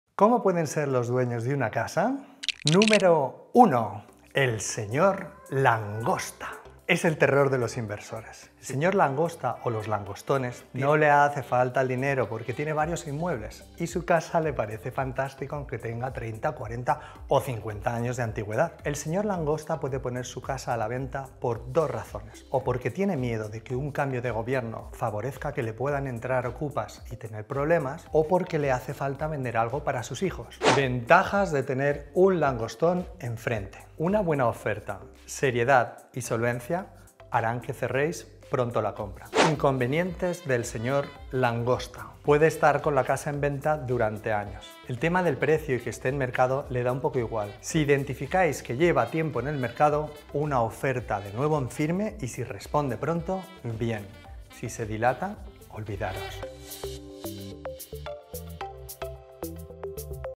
✅ 01 El SR. LANGOSTA 👉 Tipos de Vendedores 😎
📢🟣Hoy os hablamos del Sr. Langosta, o los langostones, uno de los tipos más comunes en el mercado inmobiliario.

Os damos consejos para poder negociar el precio de una vivienda con los diferentes tipos de vendedores que os podéis encontrar a la hora de comprar casa. O si os tenéis que retirar y no perder el tiempo.

Portaros bien o se os pondrán los ojos como a Rodolfo en este vídeo..😎

👉Comprar una Casa no es una decisión sencilla, por lo que os ofrecemos consejos de forma gratuita, para que esta decisión sea un poco más fácil.

📢 ADVERTENCIA: No vendemos casas. Sólo ayudamos a comprarlas 😉

💜 Luxury Angels | Buyer Agent | Madrid | Spain 💜
👉 Ángeles Nieto